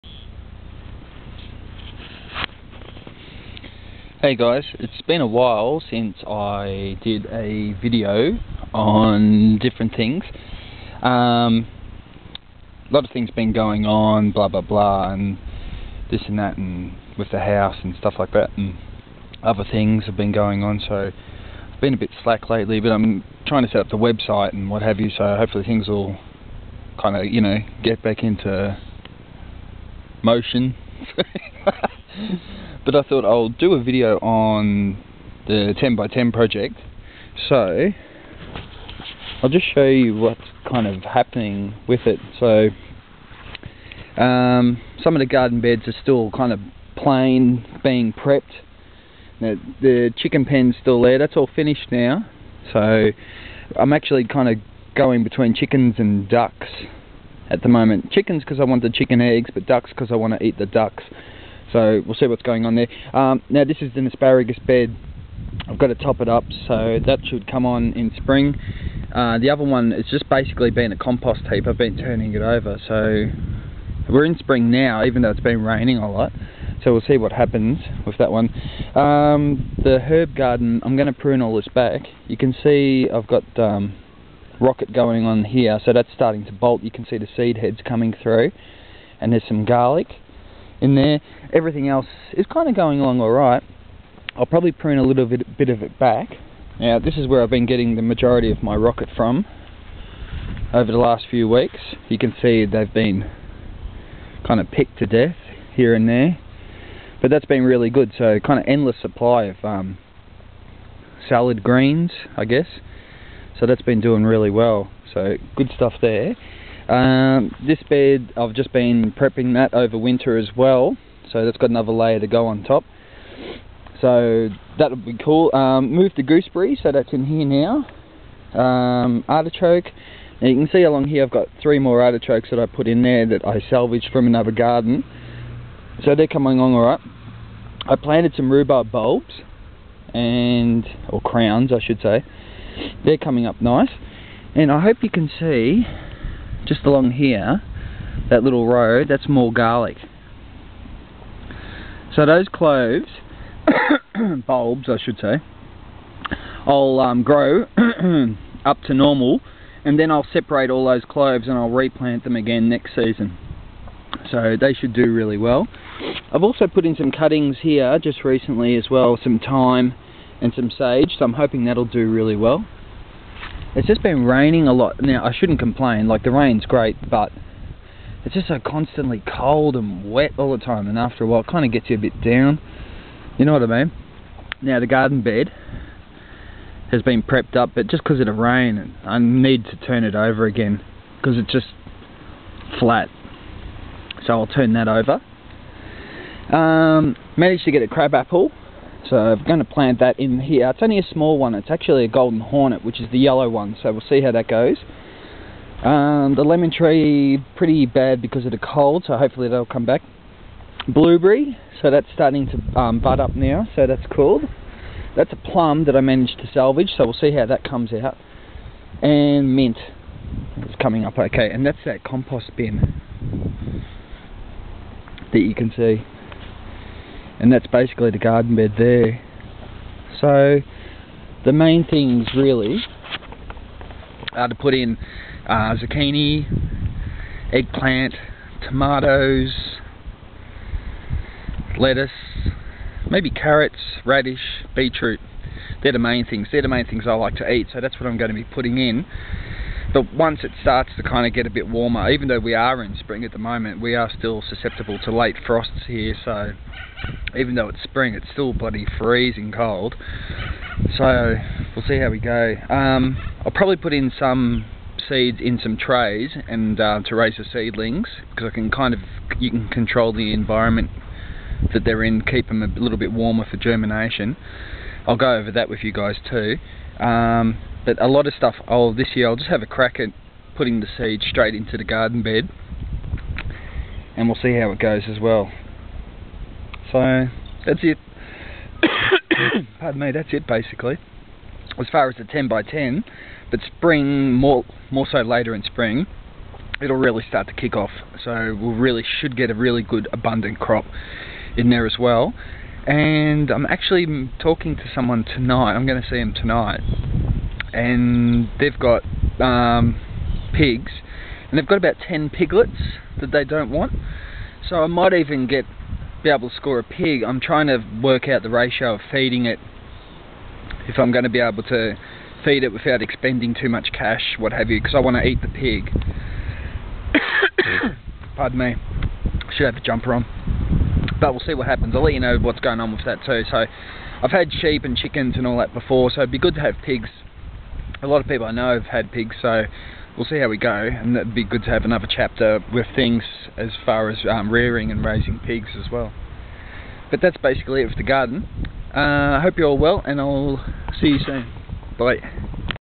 10x10 project update
im waitin for warmer weather, ive got kale, eggplant, zucchini and radish seeds in the greenhouse and im just waitin!